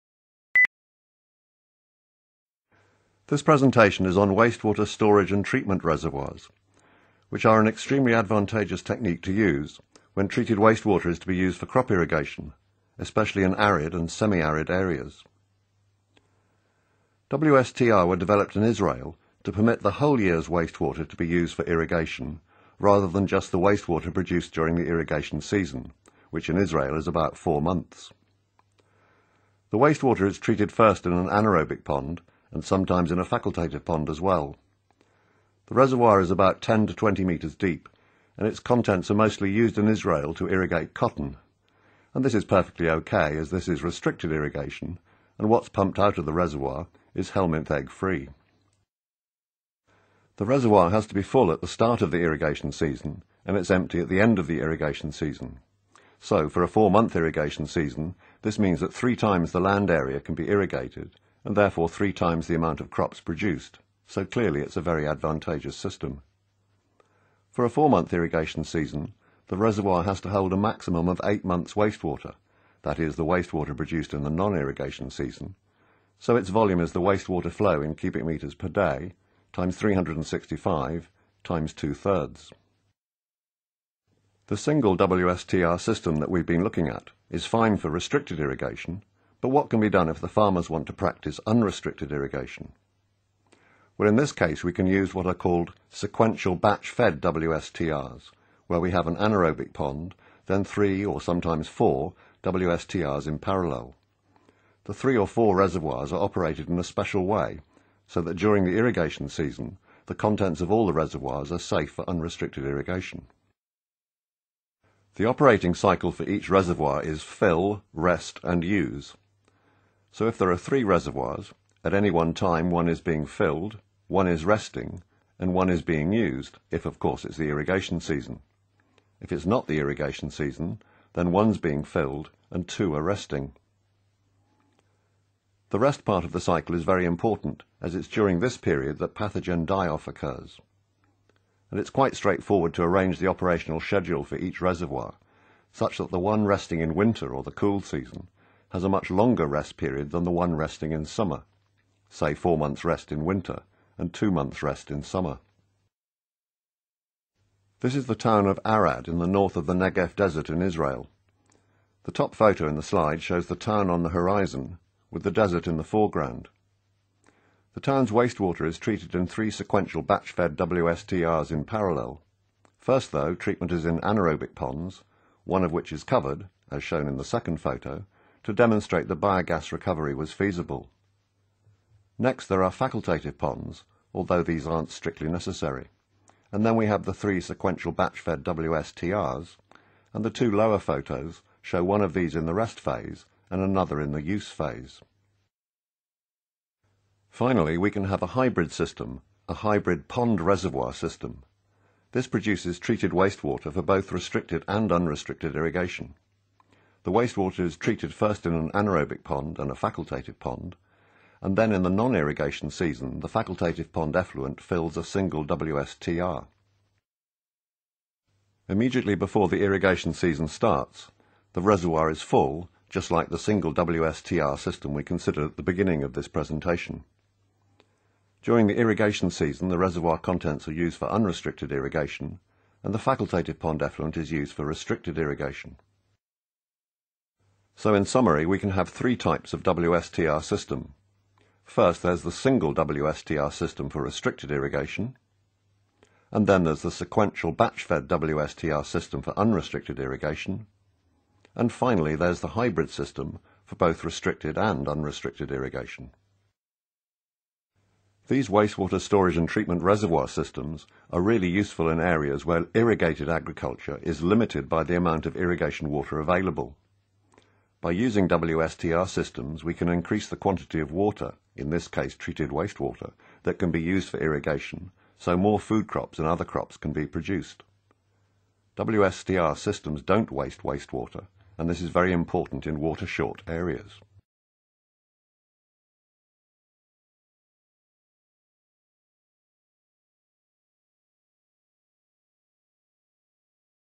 Wastewater Storage and Treatment Reservoirs
A lecture on Wastewater Storage and Treatment Reservoirs by Duncan Mara, University of Leeds, UK